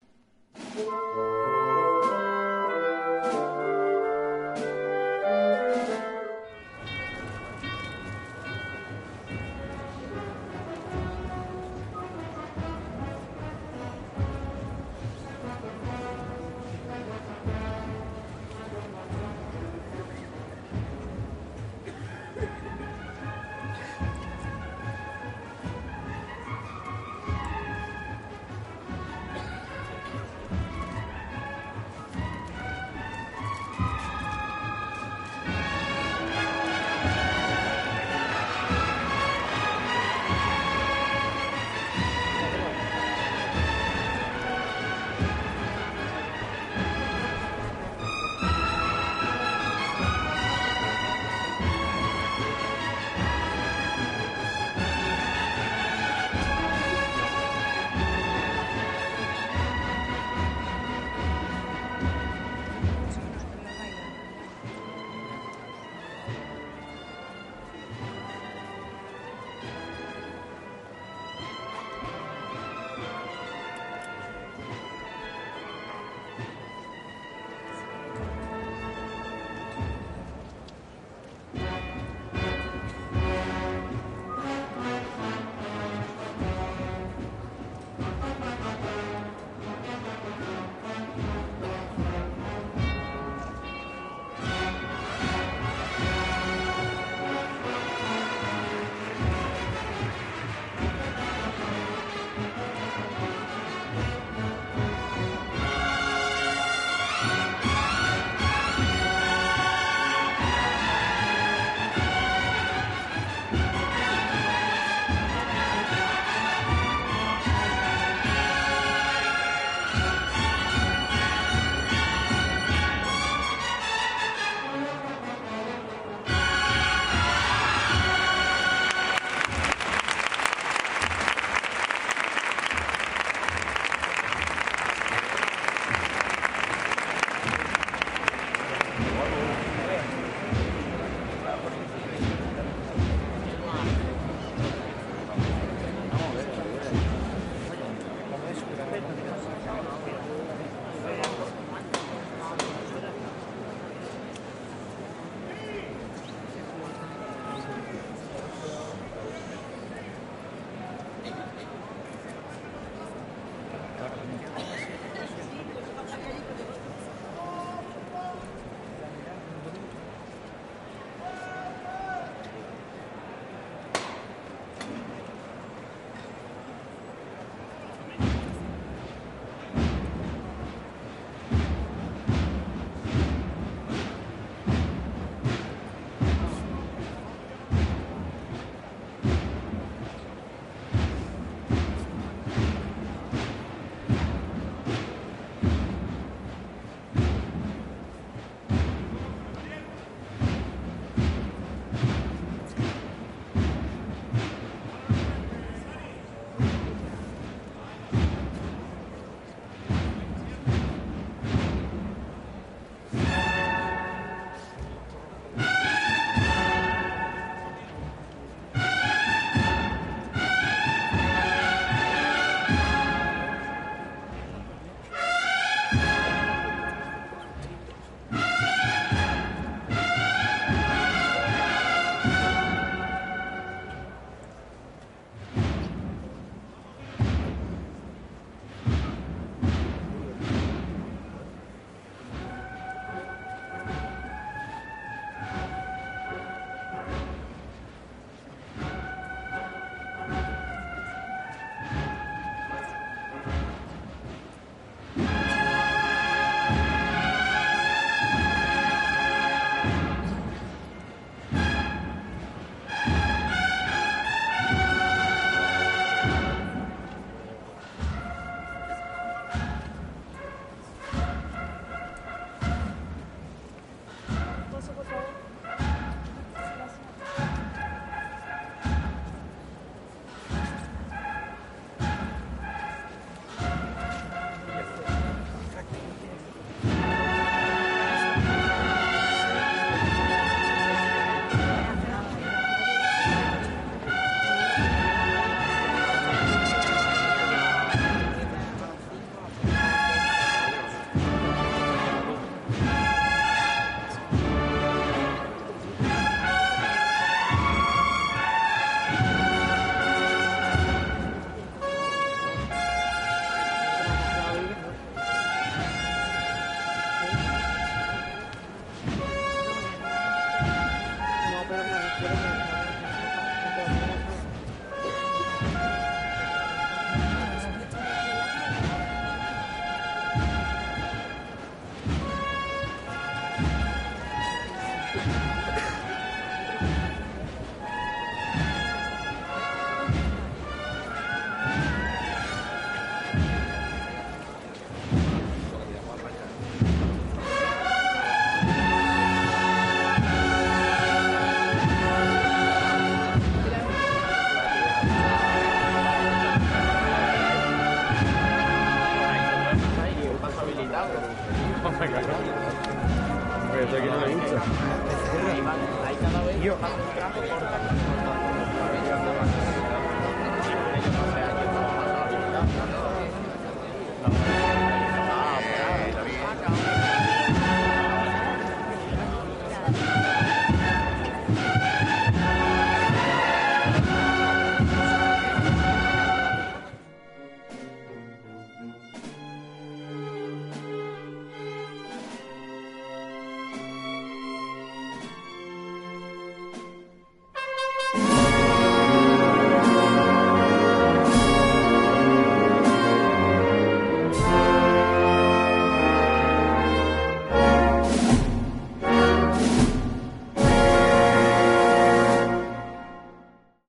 4K Santísimo Cristo de las Tres Caídas. Esperanza de Triana. Semana Santa Sevilla 2023
Santísimo Cristo de las Tres Caídas. Hermandad de la Esperanza de Triana. Semana Santa de Sevilla de 2023. 4K